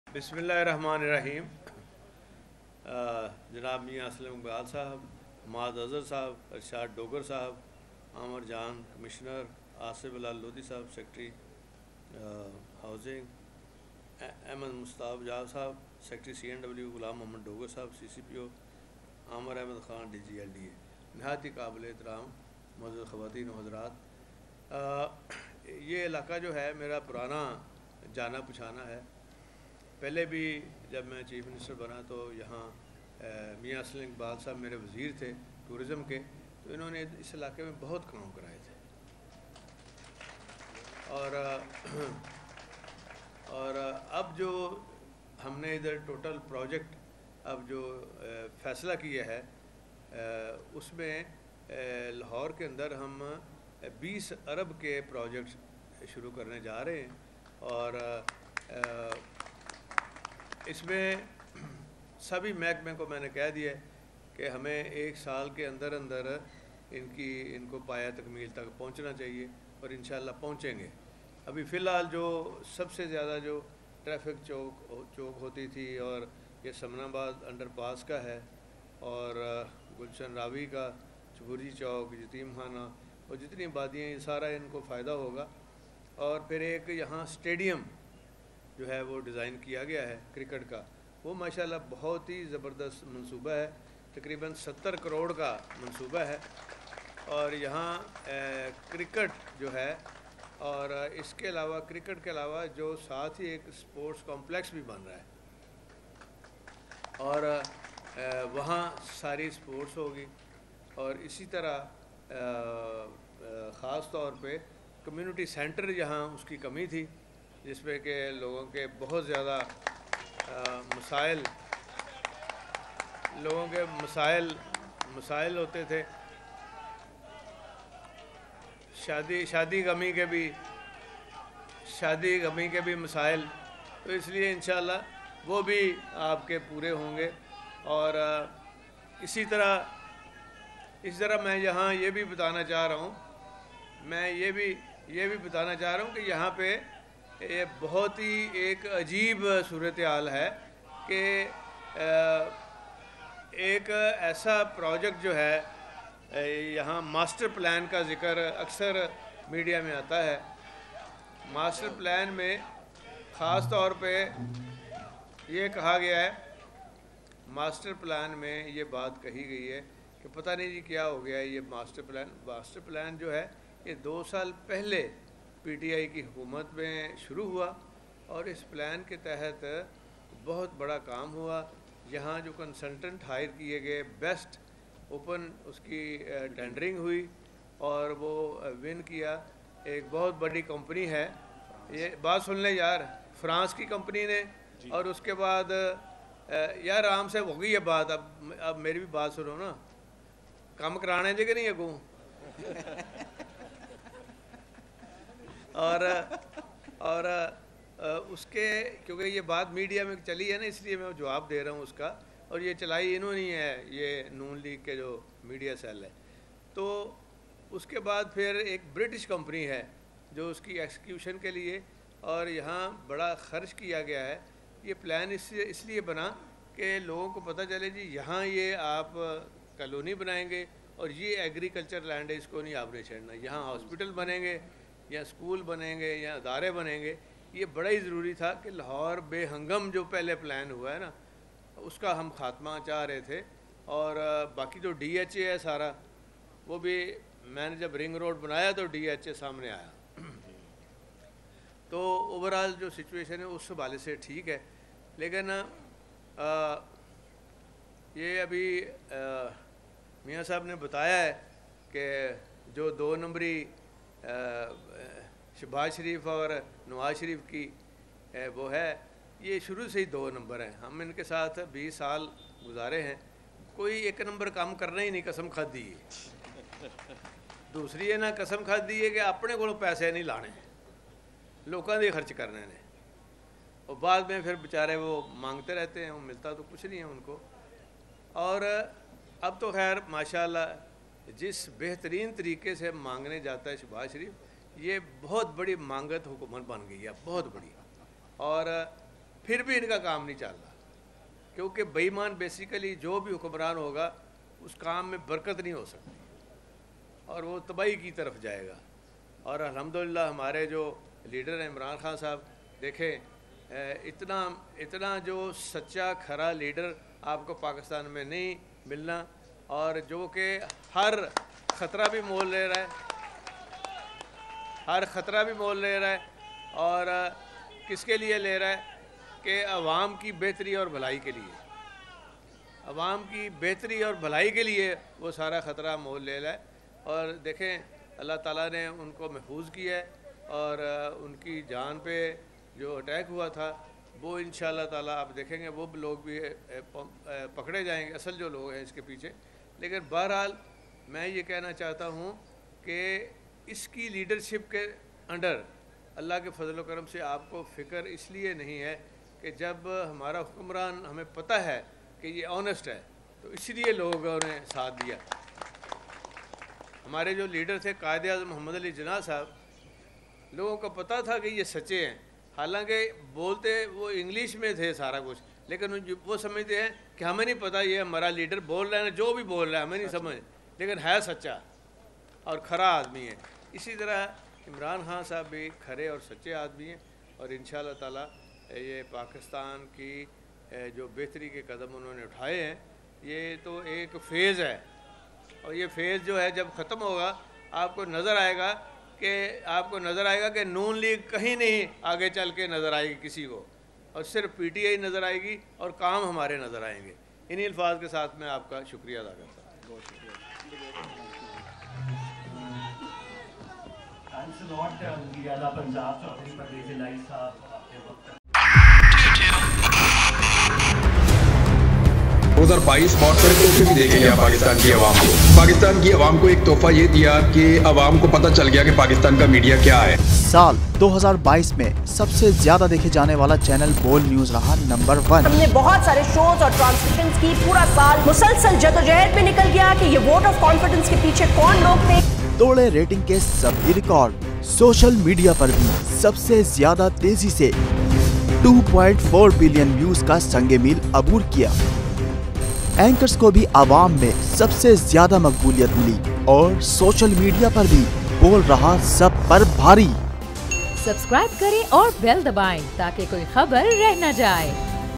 Chaudhry Pervaiz Elahi Latest Speech on BOL News | 10 JAN 2023
Development In Punjab And Lahore
No Confidence Motion In Punjab Assembly